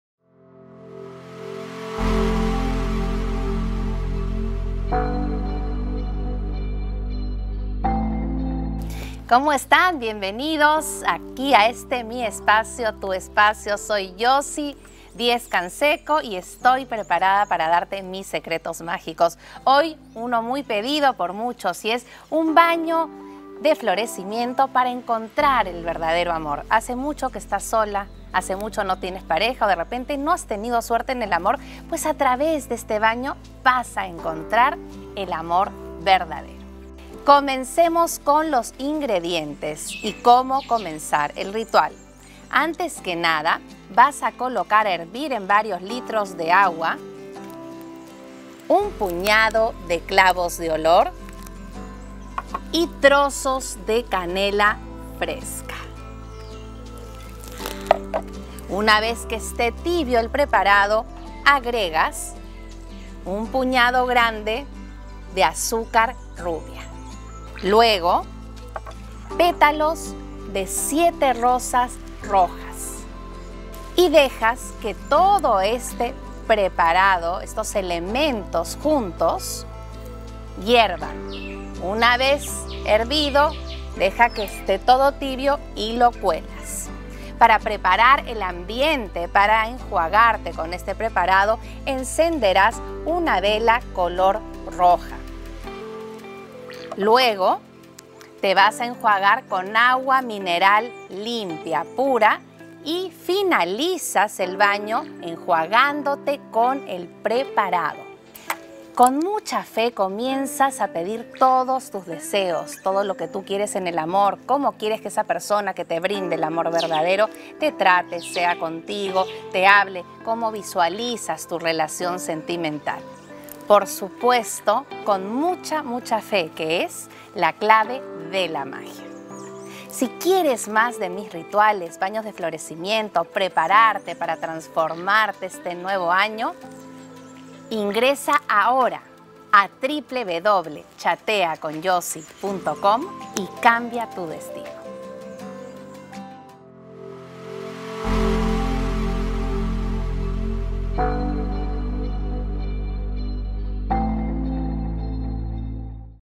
Baño de florecimiento para encontrar el verdadero amor este 2017
Hola soy Josie, aquí les dejo un baño de florecimiento para encontrar el verdadero amor este 2017.

Si quieres saber más de tu destino no dudes en chatear conmigo o mis psíquicas